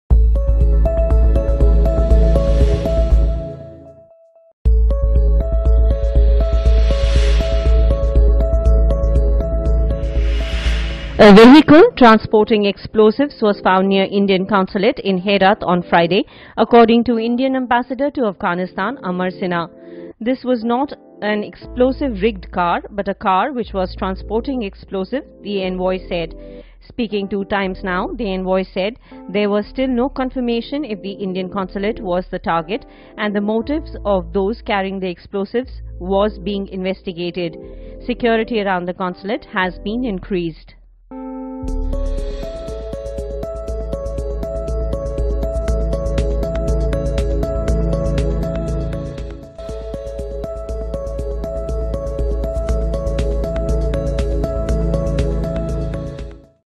Indian consulate targeted by group of armed men in Herat
The Indian consulate in Herat city of Afghanistan was targeted by a group of armed men. An explosive laden vehicle was also found outside the consulate.